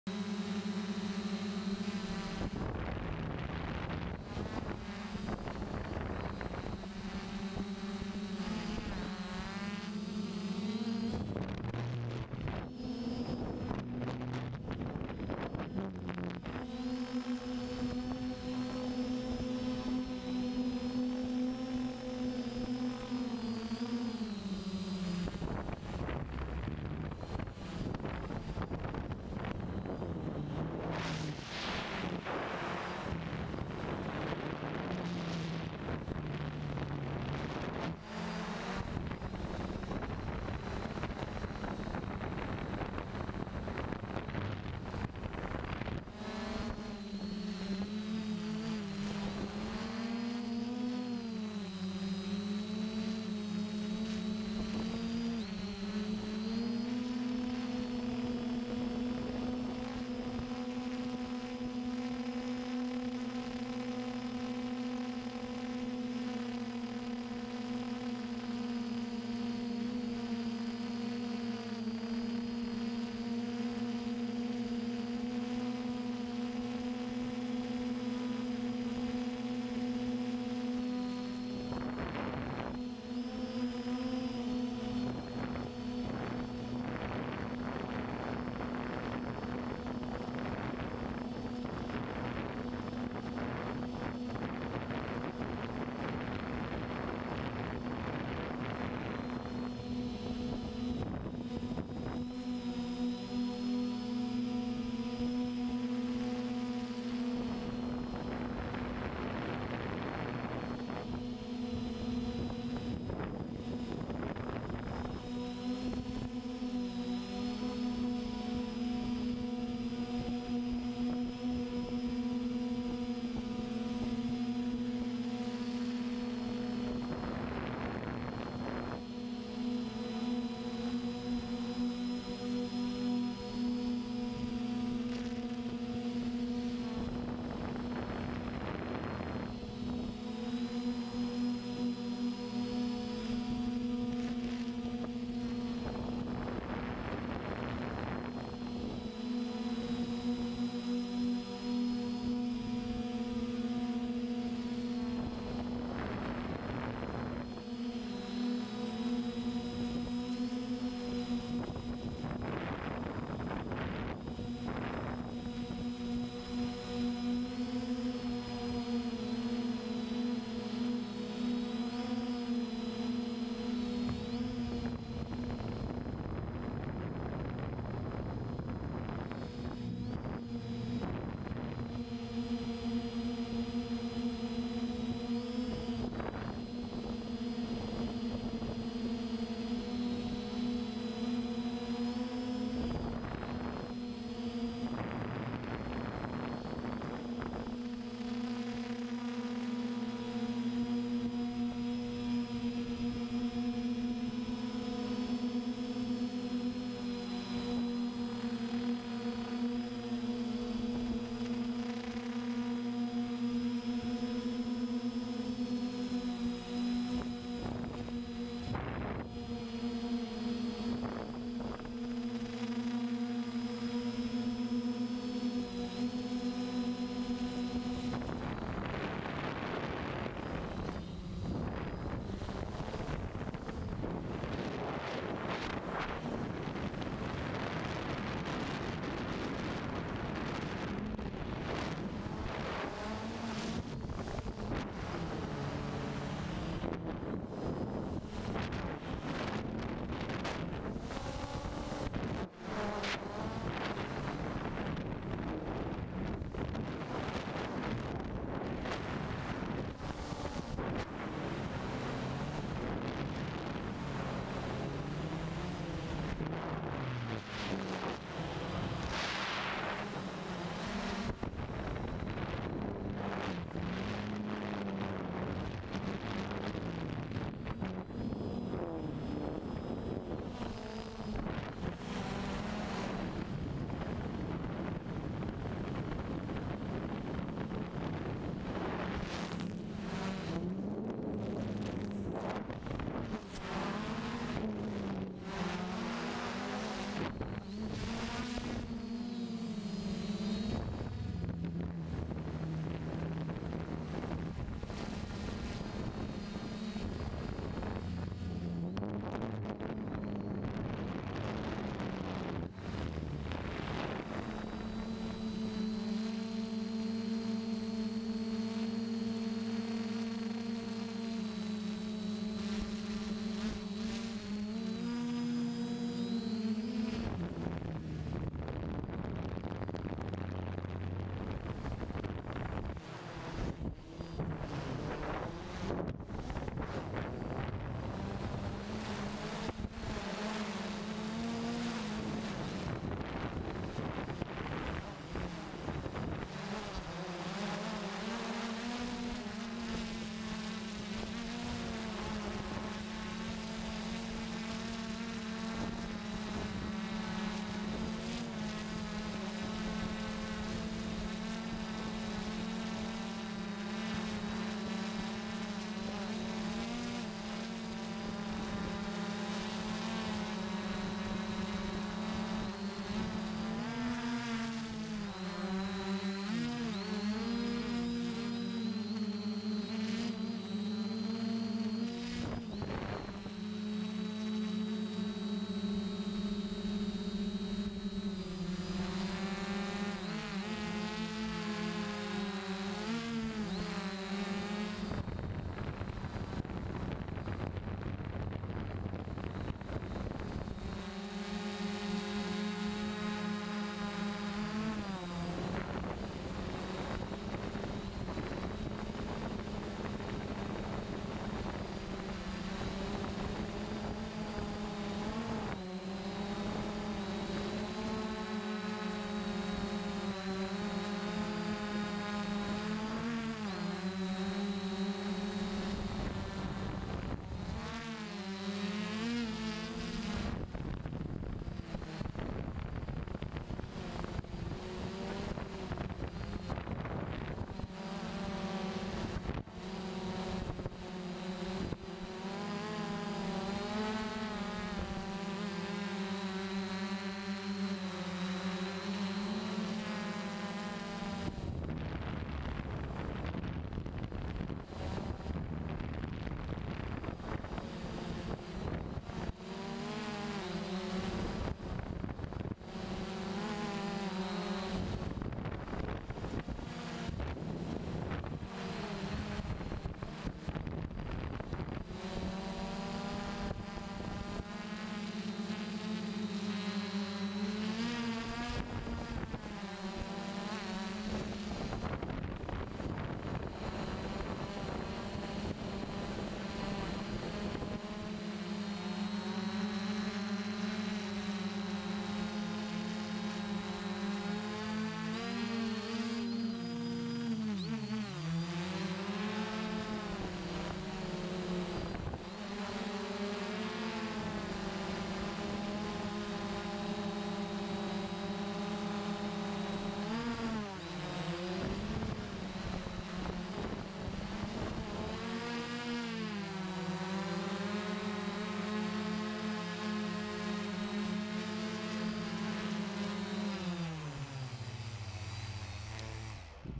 Dji F550 Manual mode with a Gimbal onboard including flips
Tested my gimbal. I knew it had it's limitations, but i didnt expect it to turn around 1 time. I also tried to find my friends Phantom up there. Not to be found as far as i can tell.